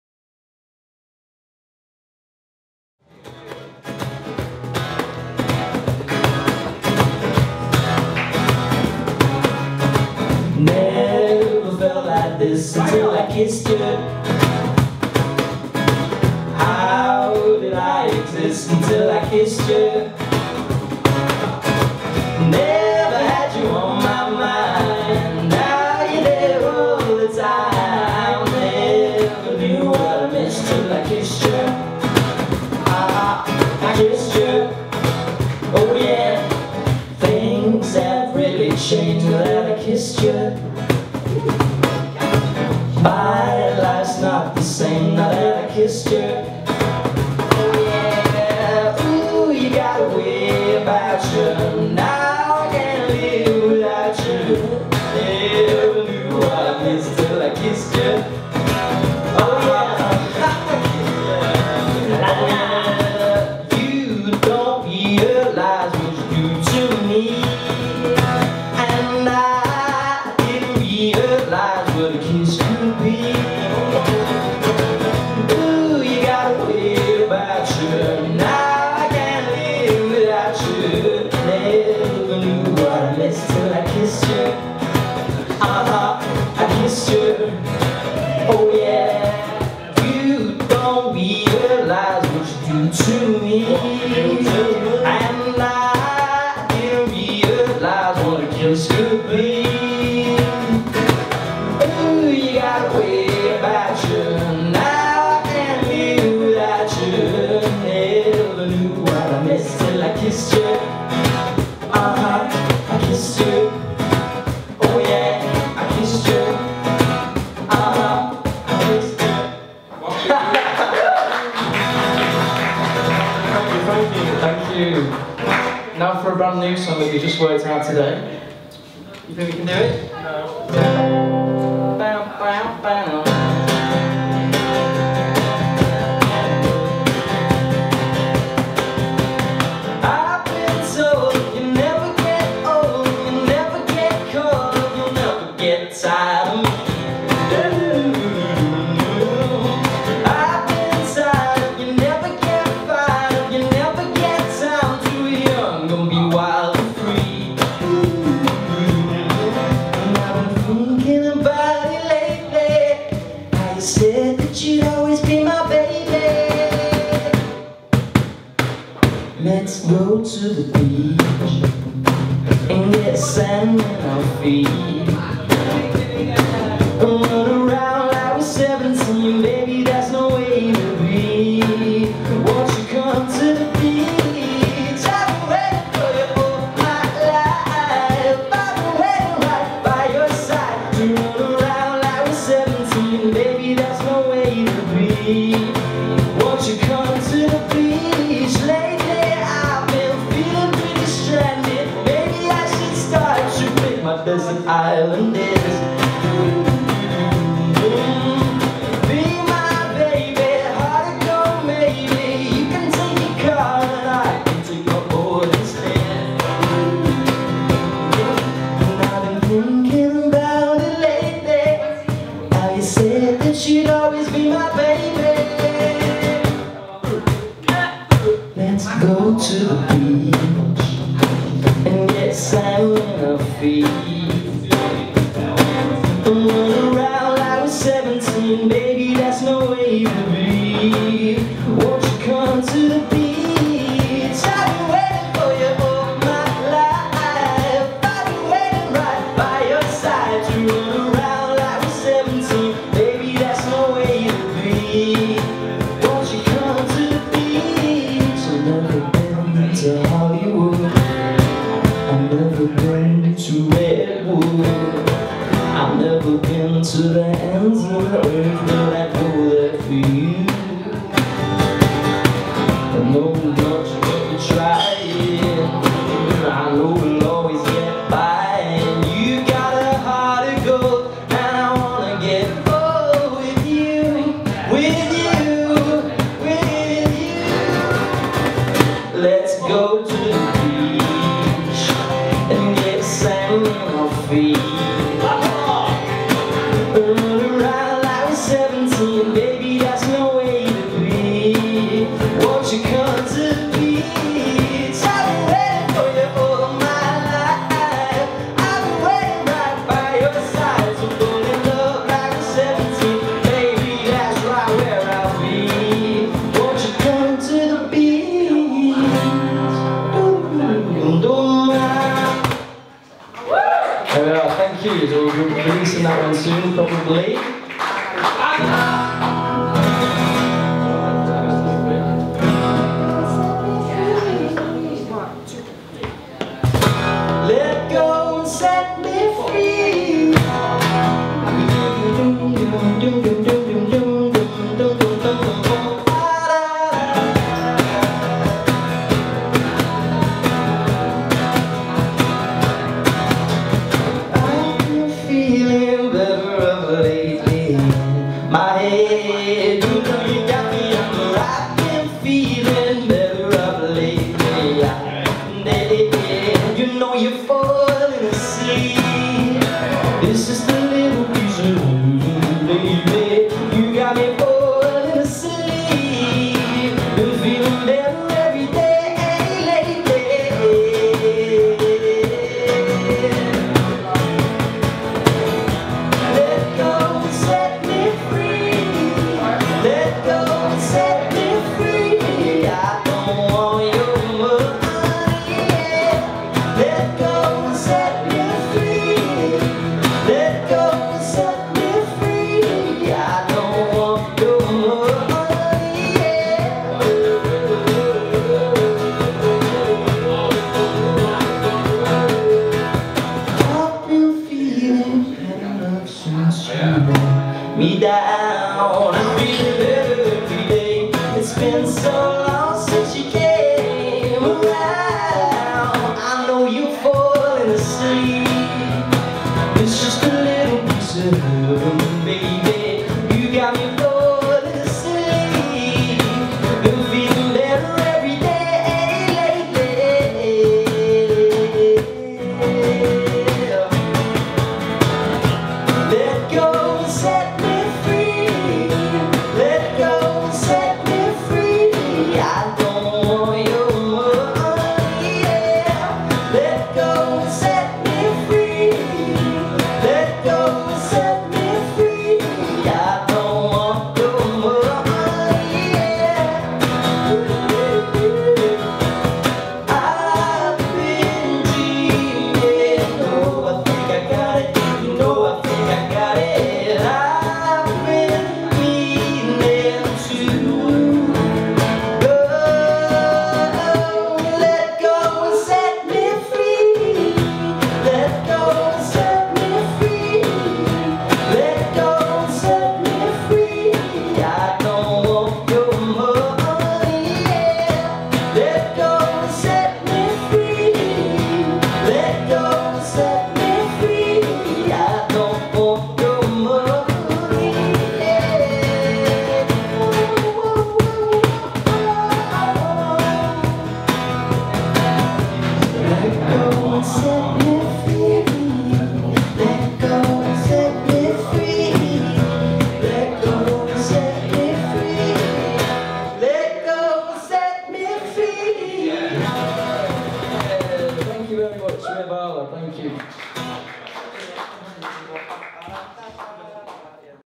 Stage & Radio - Open Mic (8/7/18) - Vala
Vala performing live at Stage & Radio's Open Mic night.

Stage & Radio hold an Open Mic every Wednesday from 6pm, for more infomation get in touch

For more information about the services we can offer at Made By Manchester, drop us a message via our Official Facebook